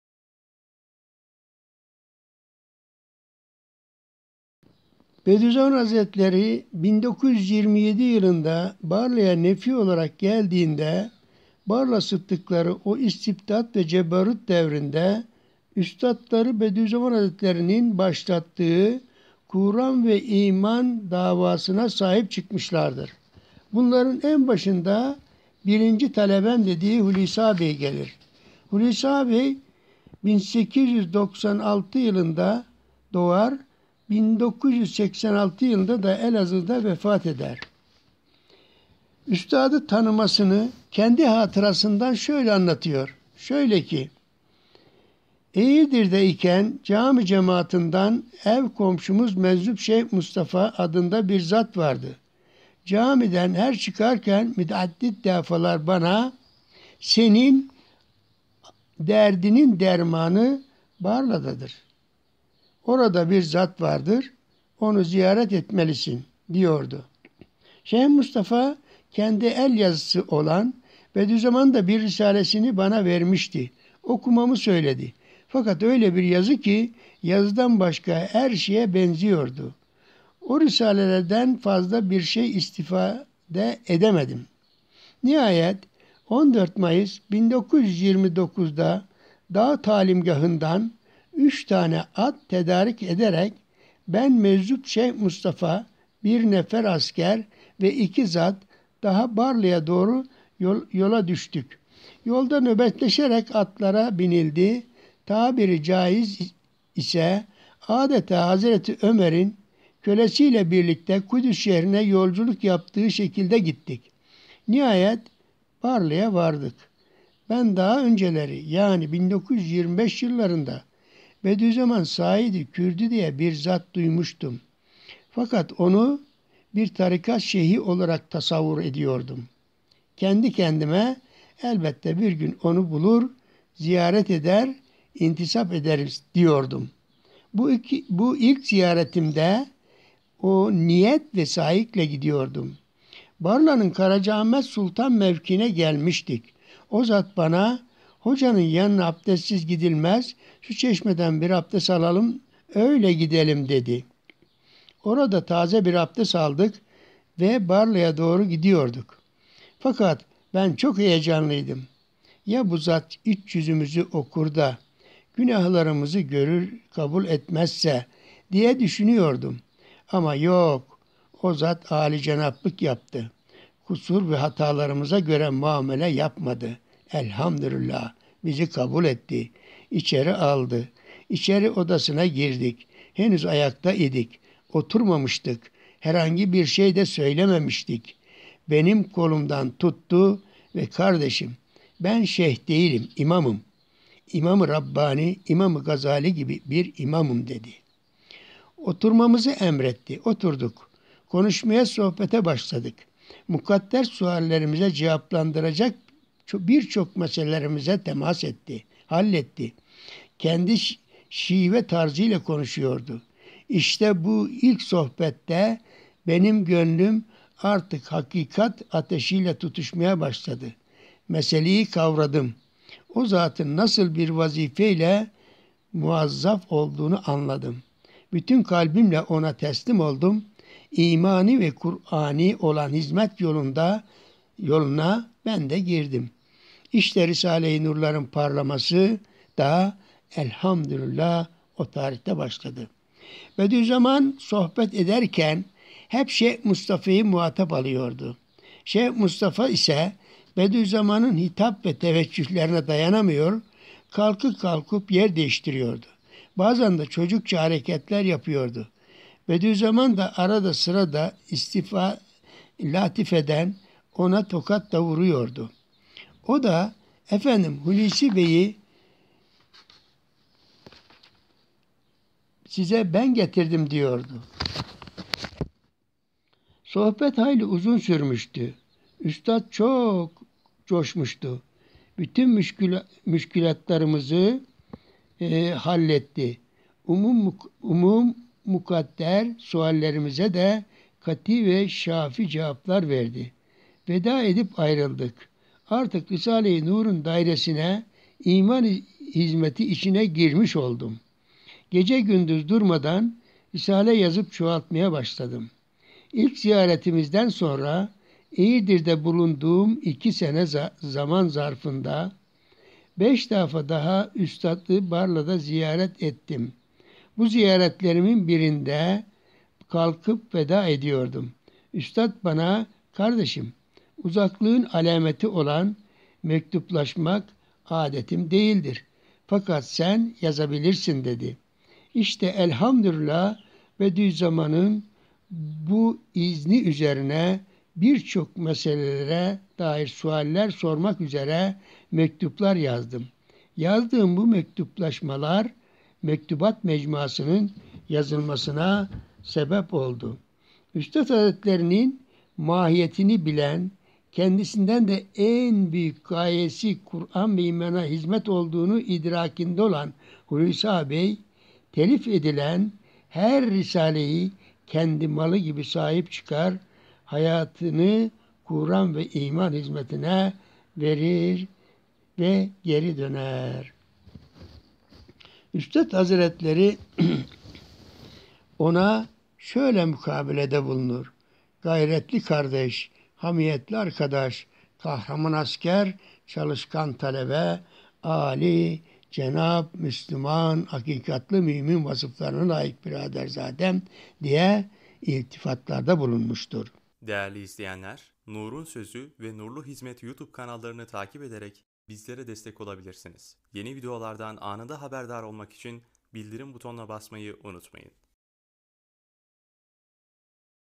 HİMMET KOÇOĞLU - BARLA SIDDIKLARI (1) - ALBAY HULUSİ AĞABEY
Bu ses kaydı telif hakkı Himmet KOÇOĞLU'na ait olup, müsaadesi alınarak yayınlanmıştır.

HİMMET KOÇOĞLU KİMDİR.

1942 yılında Denizli. Çivril kazasının Irgıllı köyünde                              doğdu. İlkokulu köyünde. Ortaokulu Dinar
kazasında bitirdi.1962 yılında SenirkentYapı Enstitüsünün inşaat Bölümünden mezun oldu. Devlet Su İşleri Kurumunda.Baraj inşaatlarında kontrolluk yaptı.1987yılında emekli oldu Isparta Kültür Eğitim Vakfının İnşaat hizmetlerinde bulundu. Bu çalışmalarına ek olarak Risale-i Nur hizmetlerinde bulunan Saffı Evvel Isparta Kahramanlarının mezarlarını bulup yaptırdı. Hayatları ve hizmetleri hakkında bilgi topladı. Isparta Kahramanları isminde iki cilt kitabı neşredildi .